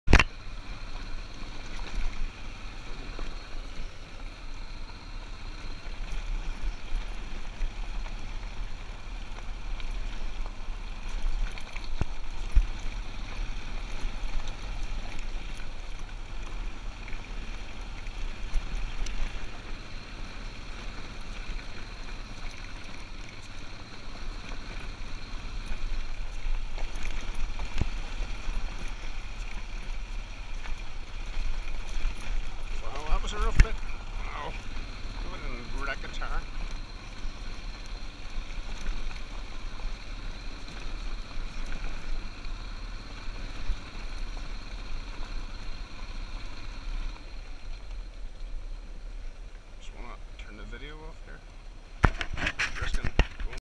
GOPR2828 Riding Up Towards Kallergi Hut From Omalos Plateau, central west Crete
Riding up the stony track to Kallergi Hut in the White Mountains (Lefka Ori), central west Crete, on a Yamaha 500cc trail bike